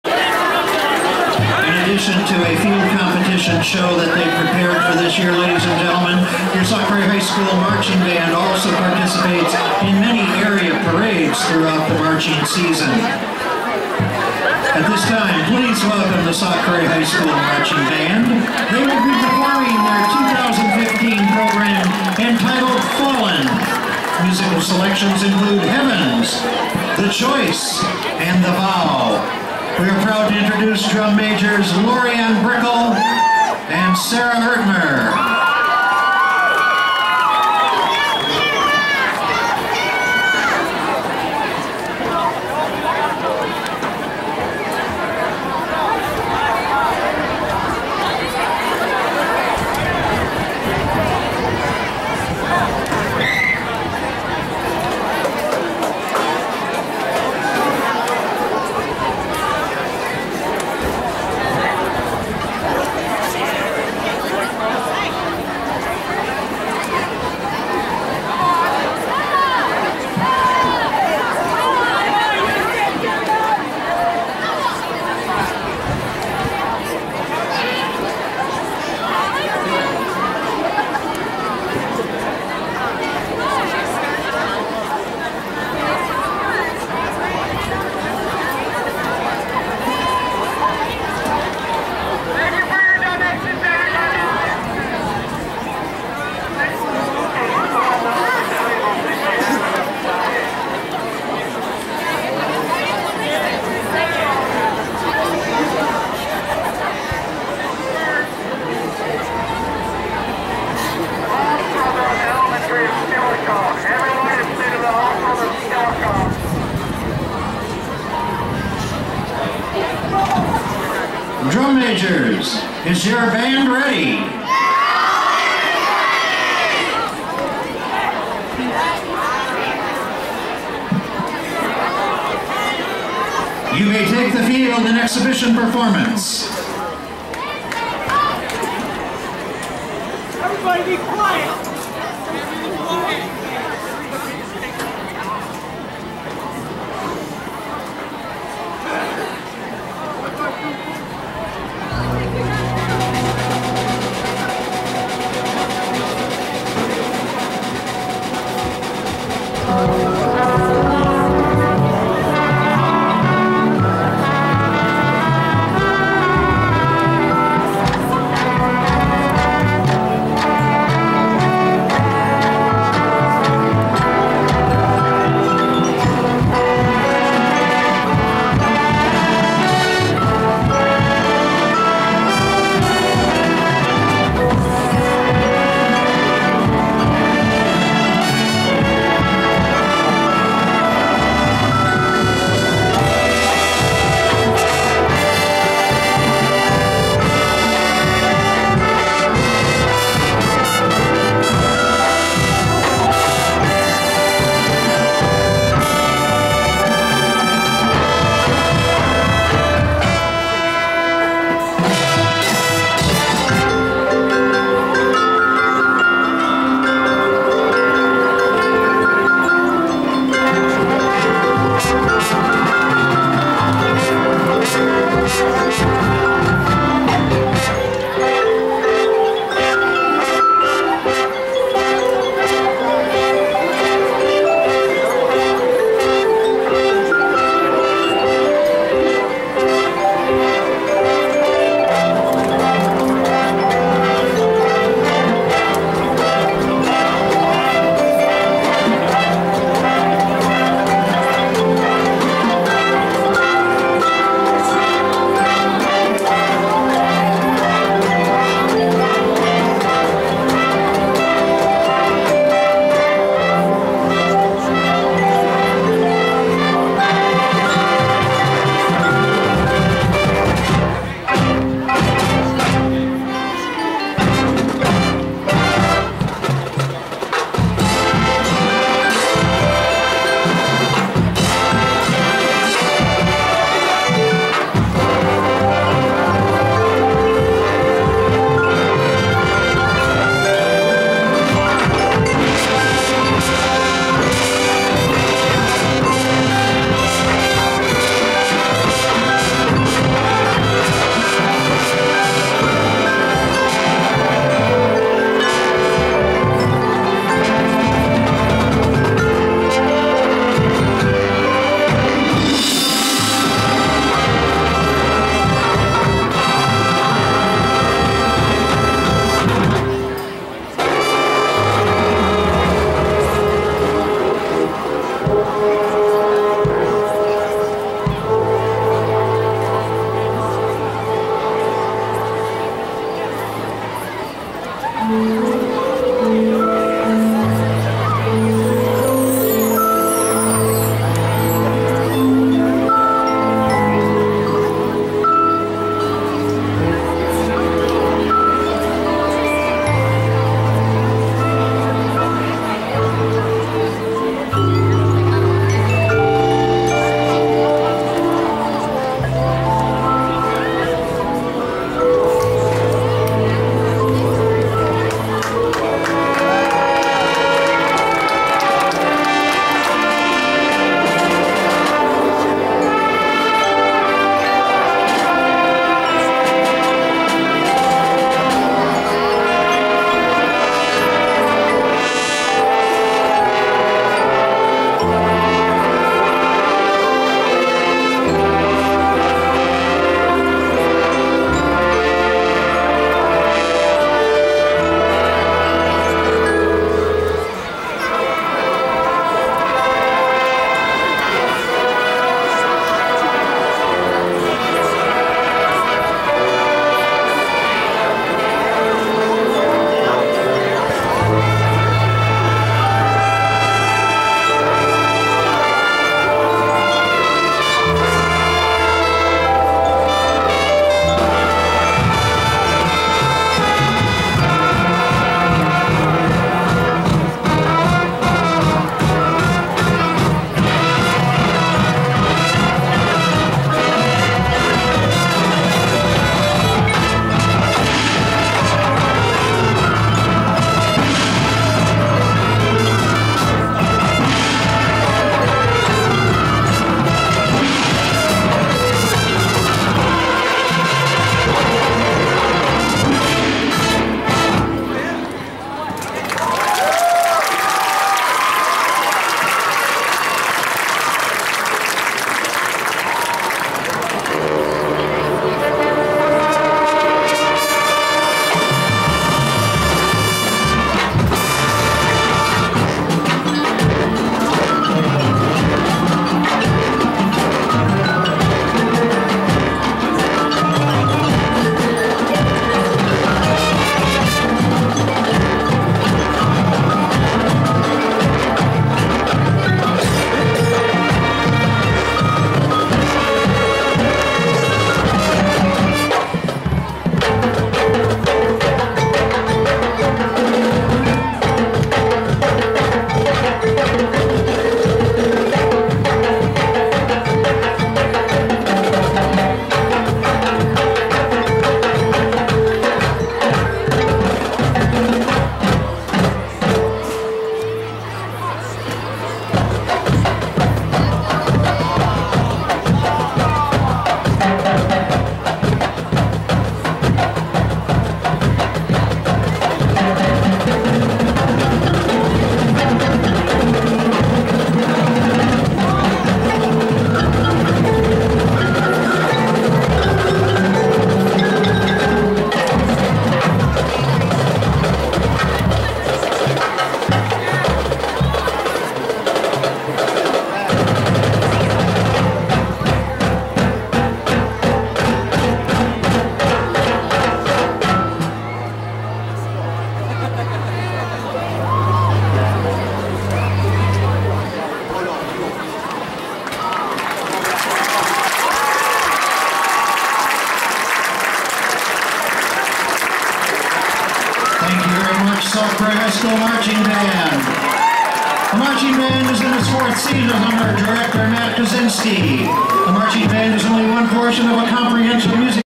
9/4/15 - Home Football Game #2
Sauk Prairie Marching Band's second performance while moving for the 2015 season.